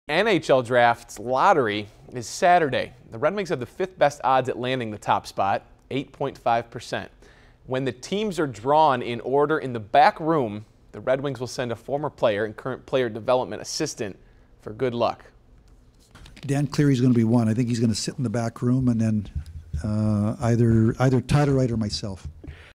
Ken Holland and Dan Cleary will represent the Red Wings at the NHL Draft
Ken Holland and Dan Cleary will represent the Red Wings at the NHL Draft. Brad Galli has more.